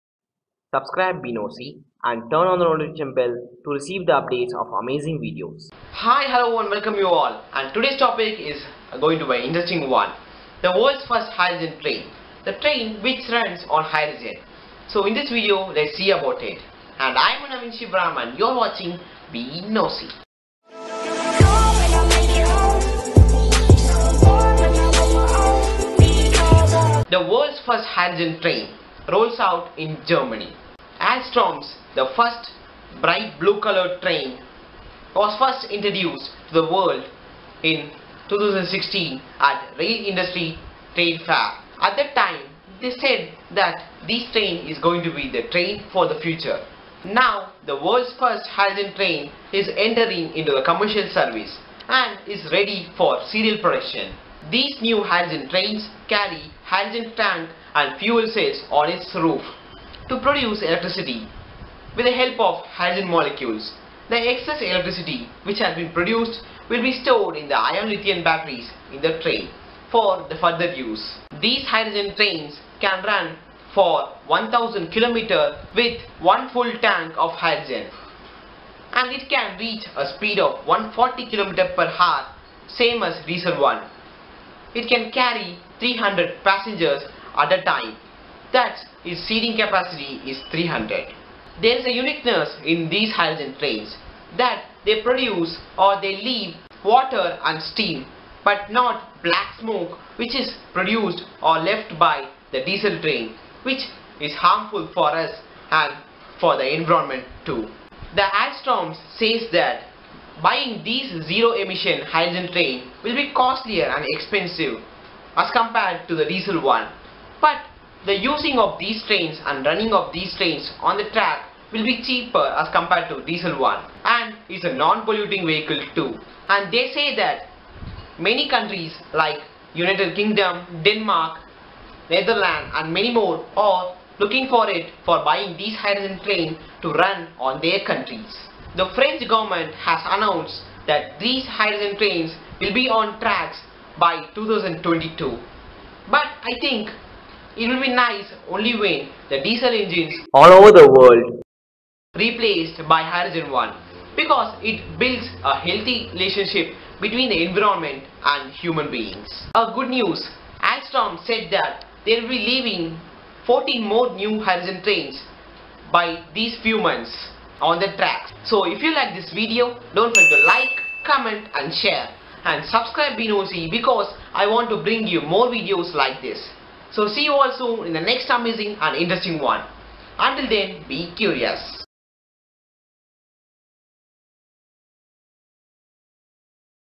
World's First Hydrogen Train - Be Nosy
The world's first Hydrogen Train By Alstom. A bright blue coloured train which runs on Hydrogen With the help of fuel cells.
So this video explains about it in Brief Of Hydrogen Train.
Watch till end to get it.
So see you all soon until then BE CURIOUS.